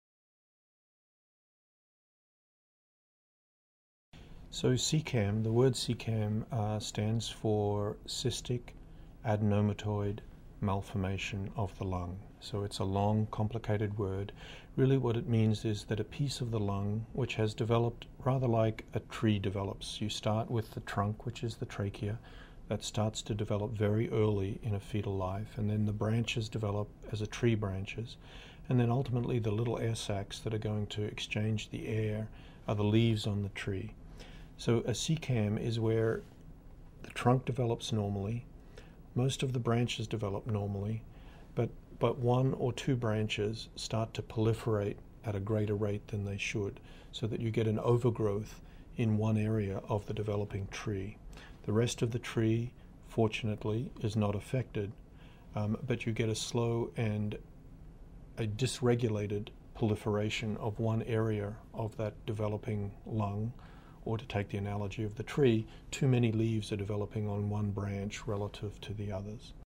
What is Congenital Cystic Adenomatoid Malformation?
Description of Congenital Cystic Adenomatoid Malformation (CCAM) and it's effects on the fetus. Presented by Sam Hawgood, MB, BS, and the UCSF Fetal Treatment Center.